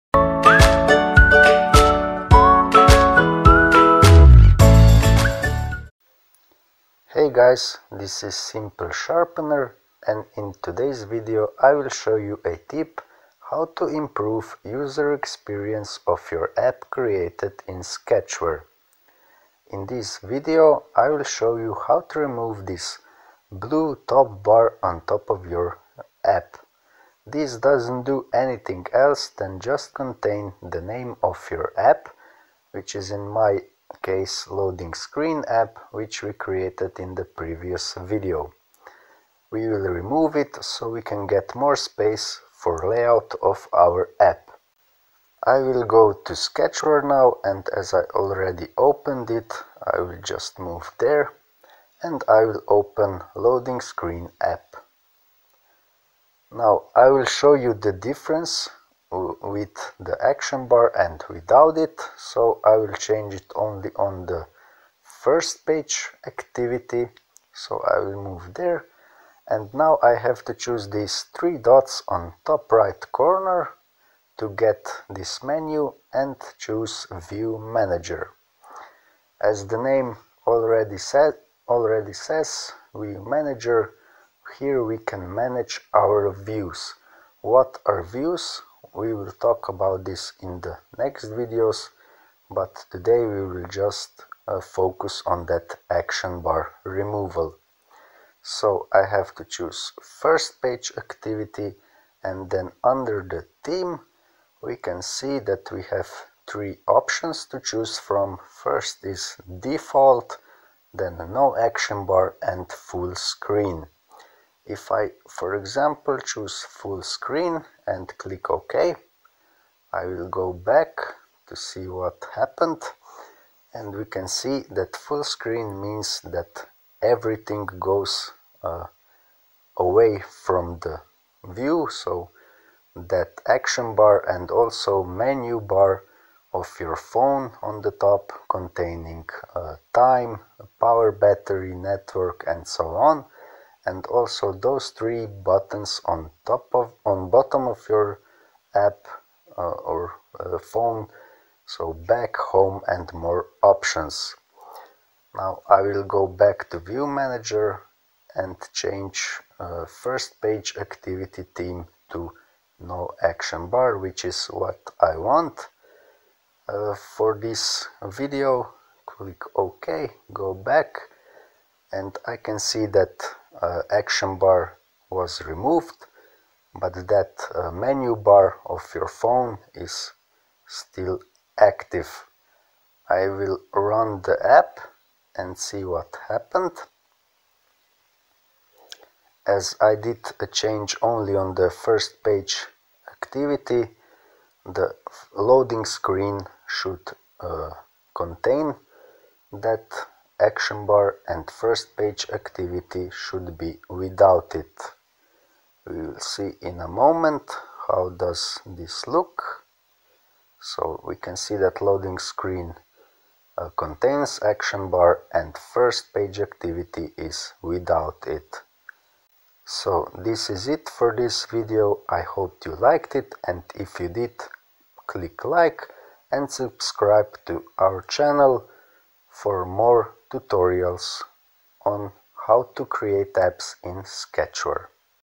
Remove ACTION BAR
Improve UX of your apps created in Sketchware by removing action bar. By doing this, you will get more space for layout of your apps.